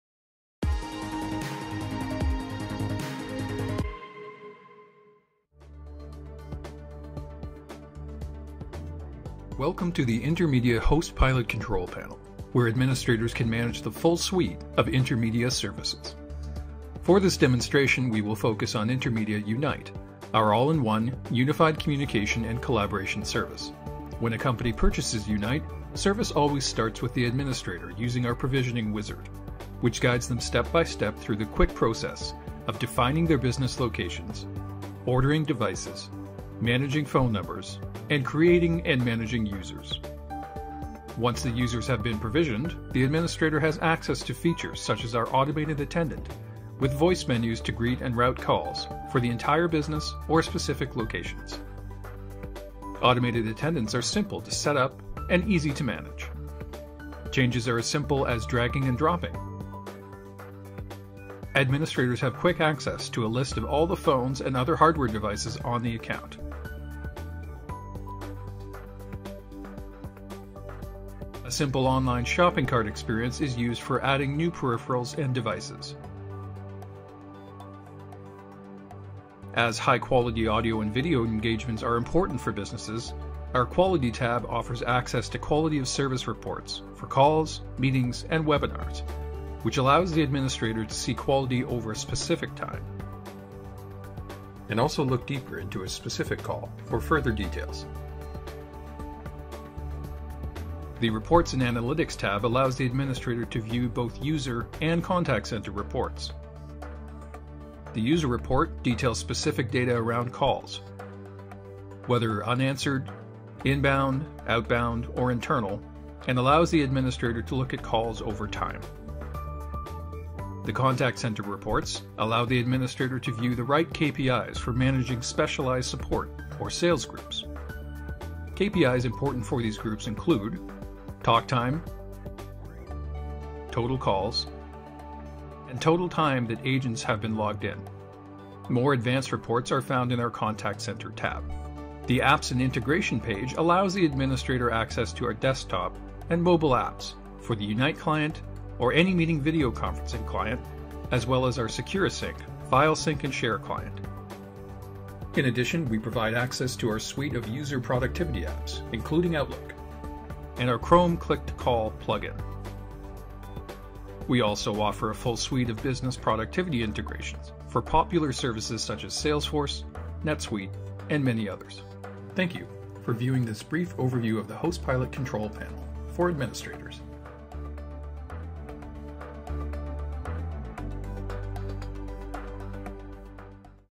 Intermedia HostPilot Demo
Video demonstration of Intermedia's HostPilot. Find out more about how to bring professional telecommunications into your business